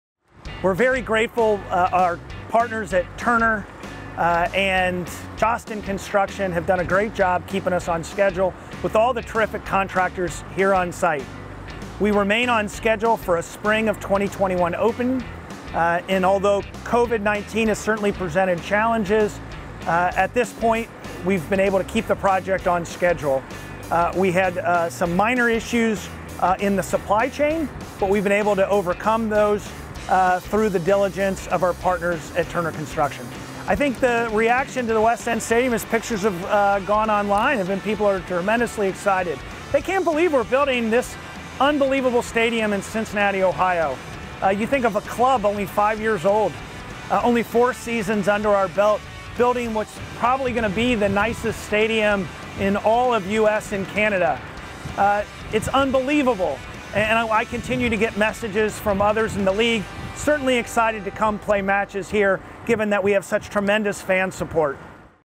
WES Update with Jeff Berding
“People are tremendously excited.” FC Cincinnati President Jeff Berding on the continued progress here in the West End.